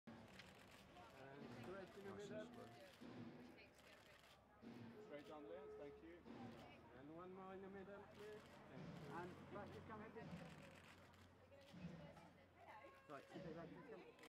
Laura Whitmore at Rankin Full Service party on July 17, 2...
Laura Whitmore at Rankin Full Service party on July 17, 2013 in London, England